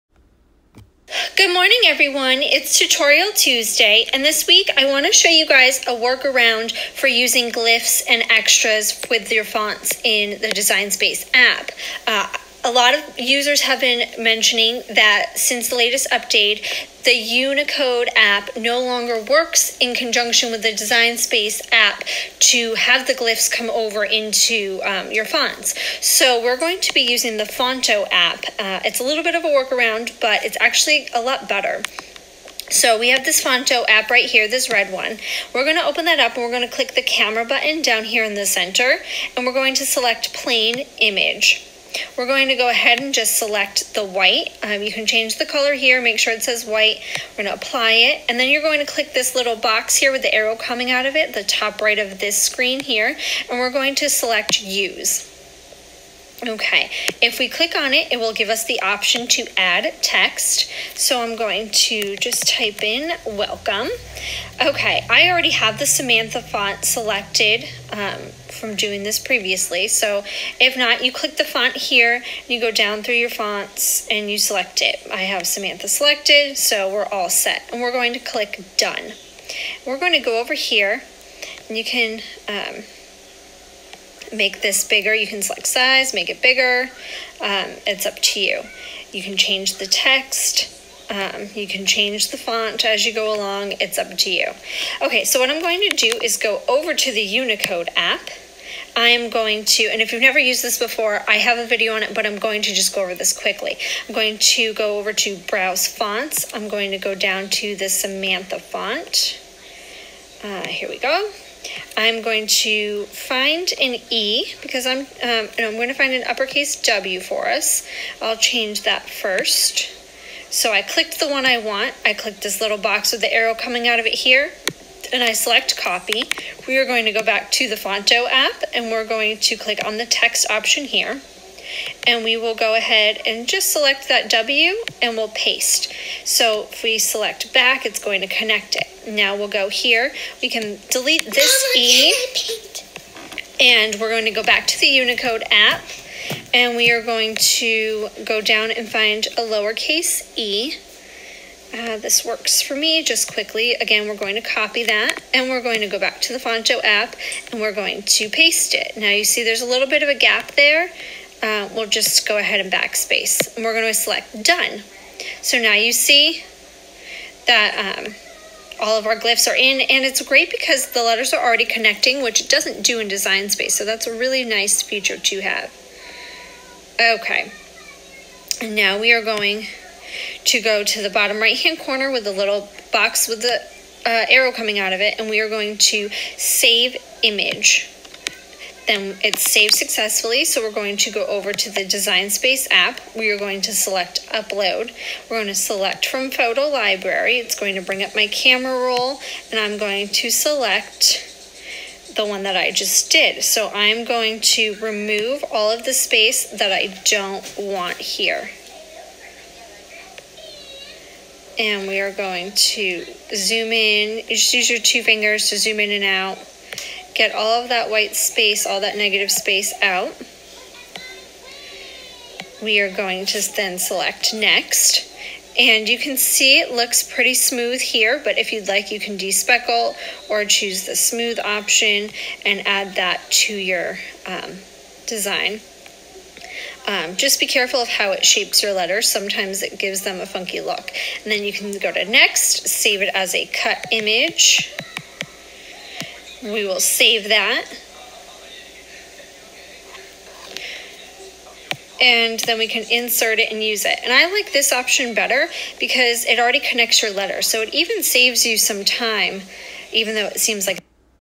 Using The Phonto and Unicode Apps for Font Glyphs with Design Space
In this video I show you how to use the Unicode app with the phonto app in order to access font glyphs and import to the Cricut design space app.

Use CATSWHOCODE for an additional discount